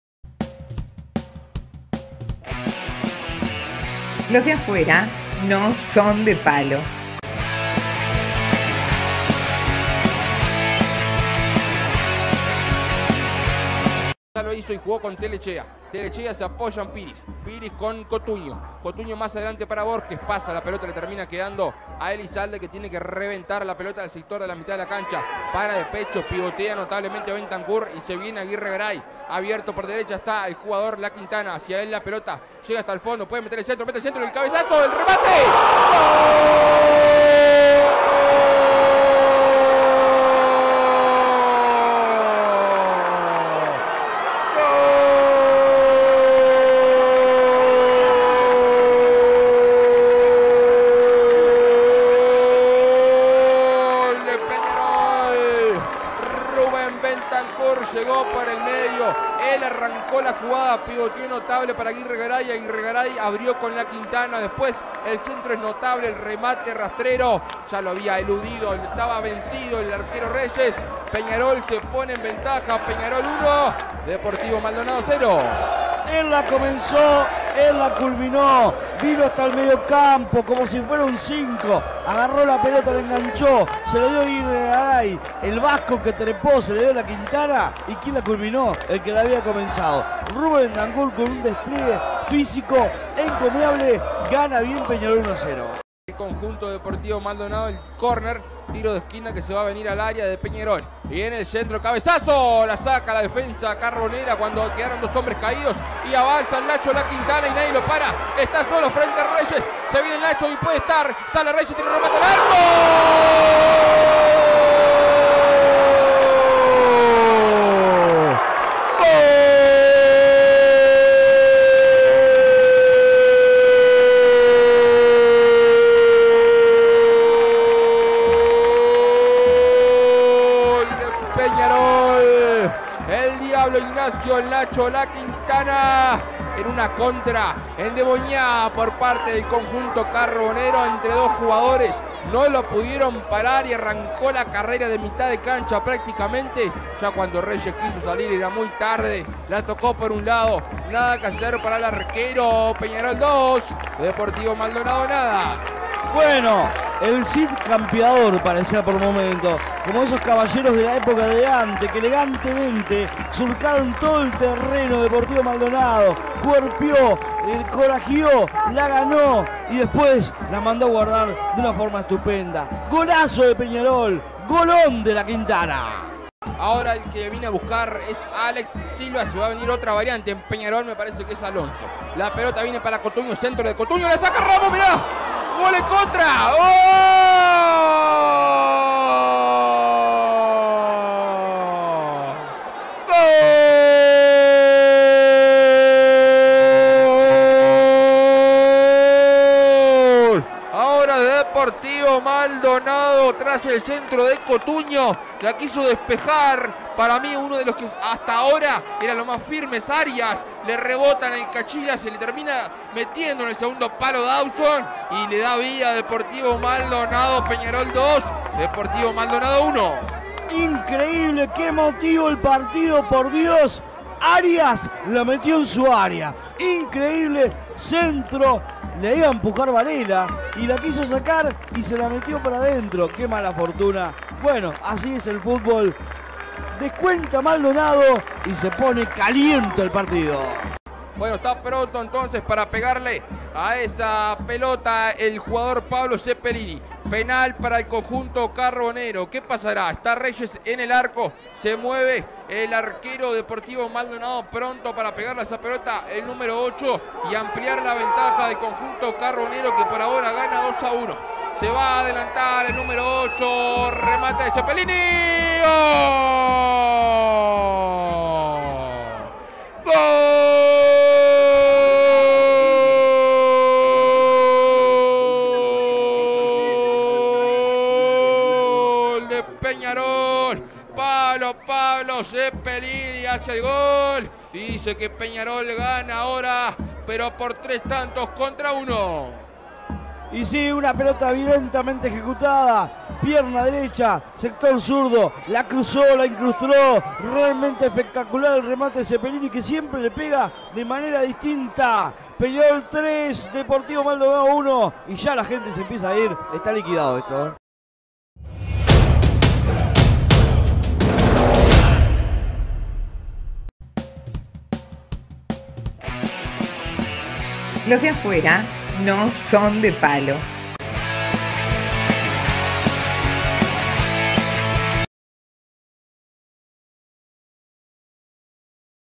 Deportivo Maldonado 1 Peñarol 3 Intermedio 2022 - (por Marcos González - LDANSDP)
Los goles del partido, por Marcos González y Gustavo Rodríguez (agradecimiento especial) de @losdeafueranosondepalo .